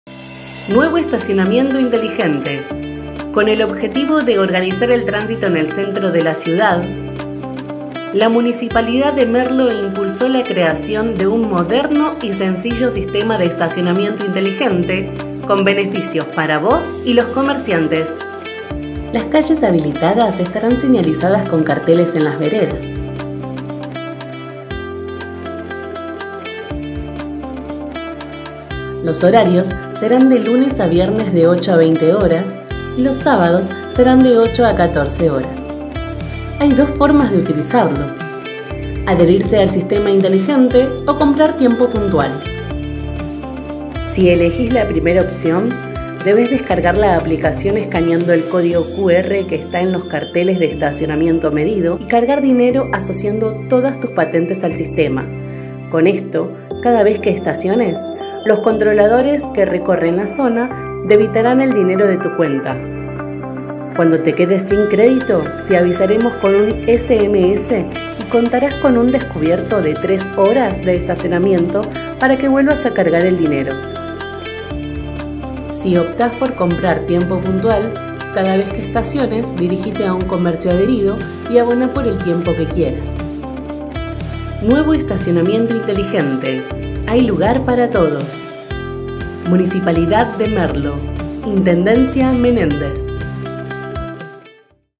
SISTEMA DE ESTACIONAMIENTO INTELIGENTE EN MERLO
La Subsecretaría de Tránsito de la Municipalidad de Merlo lanzó un nuevo sistema de estacionamiento inteligente. Conocé más en este video.